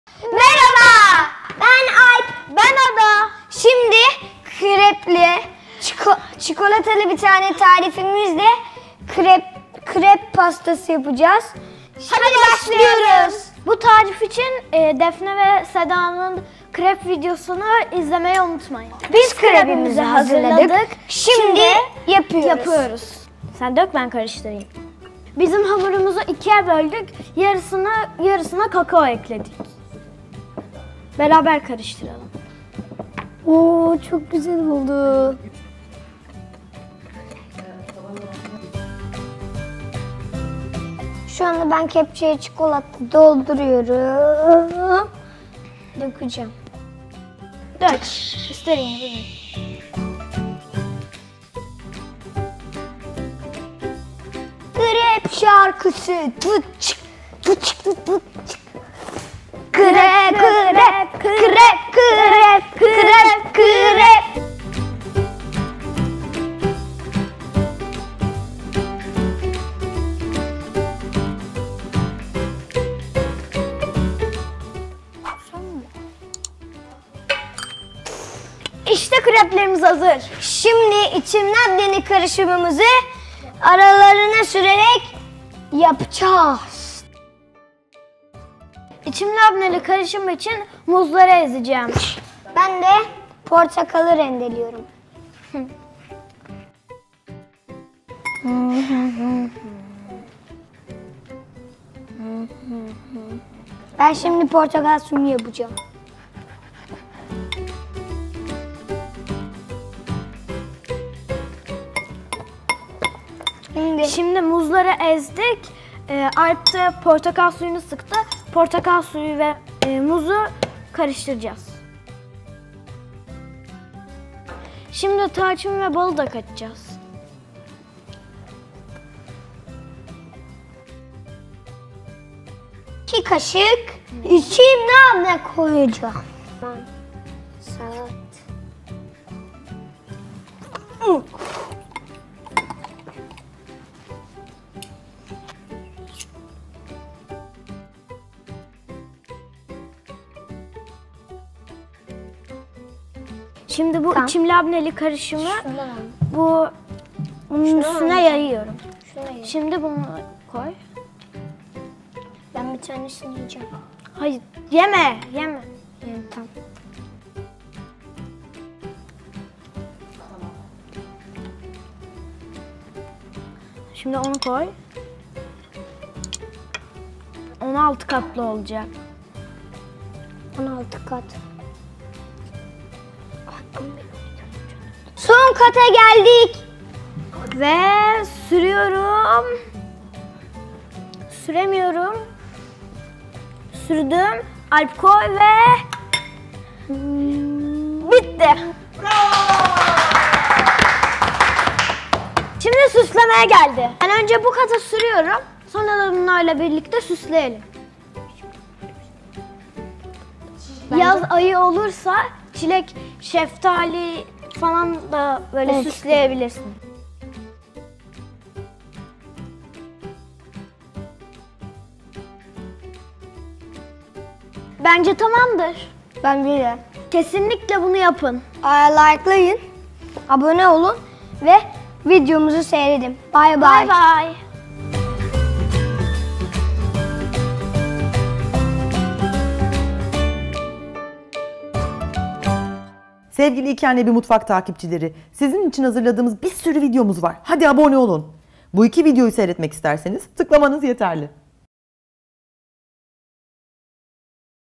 Krep Pasta (2 Yaş+)
Doğumgünleri için şekersiz, çok lezzetli ve çocuklarla yapması çok kolay bir pasta yaptı Ada ve Alp; KREP PASTA. İçim Labne'li sosu da katlarına sürdüler. Hadi Mutfağa!
Malzemeler:
Krep hamuru için (yaklaşık 15 adet krep) – 200 gram (1.5 su bardağı) tam buğday unu, 4 yumurta, 500 ml süt, 2 yemek kaşığı kakao, 1 tatlı kaşığı tereyağı
Kreması için – 200 gram İçim Labne, 1 olgun muz, 1 yemek kaşığı bal, ½ çay kaşığı tarçın, 1 portakalın kabuğunun rendesi, 1/2 portakalın suyu, 2 yemek kaşığı nar tanesi, 1 yemek kaşığı dövülmüş ceviz
Nasıl Yapılır?
1. Önce krepleri hazırlayın; tam buğday ununu kuyu şeklinde açıp çırptığınız yumurtaları içine dökün ve karıştırın.
2. Sütü yavaş yavaş dökerek çırpın.
3. Vaktiniz varsa yarım saat kadar buzdolabında dinlendirin.
4. Tavaya biraz tereyağı ekleyip eritin ve krep karışımına ekleyip karıştırın.
5. Krep hamurunu ikiye bölerek birine kakao ekleyin ve çırpın.
6. Isınan tavaya 1 kepçe kadar (tavanızın boyutuna göre) koyup rengi değiştikten sonra ters çevirin.
7. Tavadan alıp kakaolu ve sadeleri iki ayrı tabakta üstüste koyun.
8. Krepleri hazırladıktan sonra, bir kasede İçim Labne, muz, bal, tarçın, portakal kabuğu rendesi ve portakal suyunu karıştırıp mikser ile iyice çırpın.
9. Bir krebi servis tabağına koyup üzerine ince bir kat İçim Labneli karışımdan sürün.
10. Kalan krepleri de aralarına İçim Labneli karışımdan sürerek üst üste koyun.
11. En son krebi de koyduktan sonra kalın bir kat İçim Labneli karışım koyup nar ve dövülmüş ceviz ile süsleyin. Miss, afiyet bal olsun.